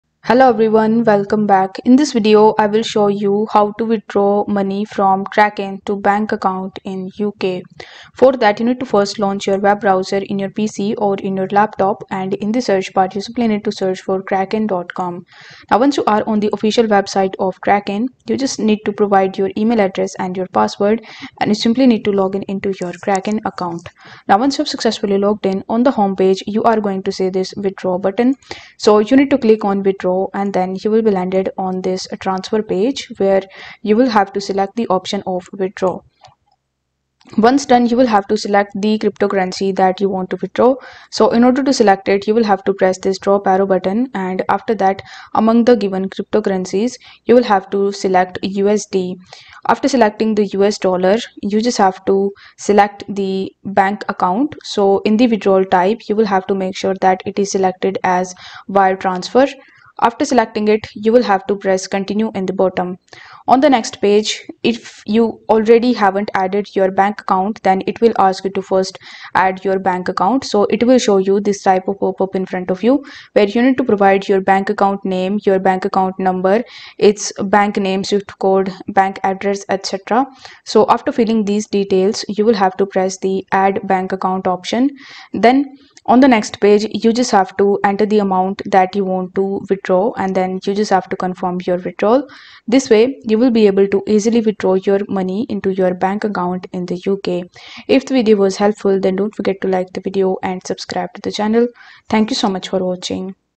How to Withdraw Money From Kraken to Bank Account UK 2024
Welcome to Token Tutorials! In each video, I will show you How to Withdraw Money From Kraken to Bank Account UK 2024